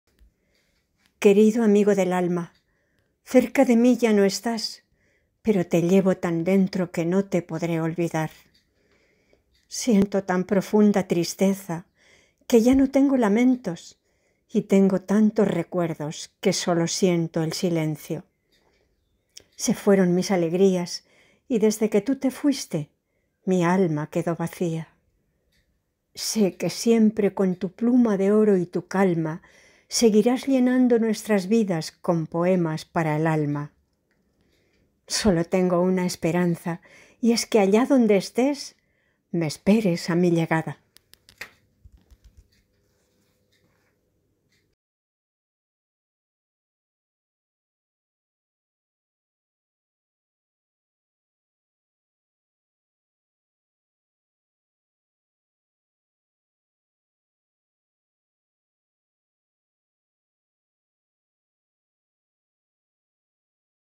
Amigo del alma de Antonia Barra en la Librería Anticuaria El Laberinto de Córdoba
Vídeo para el poema "Amigo del alma" de Antonia Barra interpretado por ella misma y dedicado a las personas fallecidas por la pandemia. Imágenes de la Librería Anticuaria El Laberinto de Córdoba tomadas en agosto de este año, salvo las de la escritora e intérprete Antonia Barra que fueron tomadas en el Café Central de Madrid.